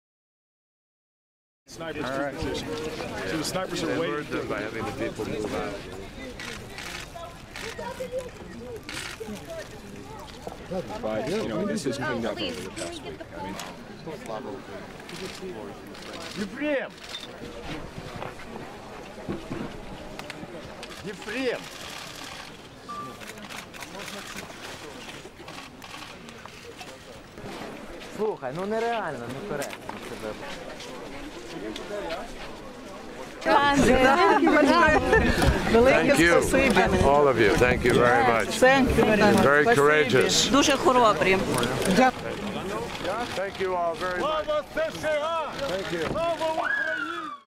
John Kerry paid tribute to fallen Ukrainians in Kiev
UKRAINE, Kiev : US Secretary of State John Kerry arrived in Kiev Tuesday for talks with Ukraine\'s new interim government, and paid his respects to the victims killed during clashes in Independence Square. Read the full story here.

This video was originally published on 5 March 2014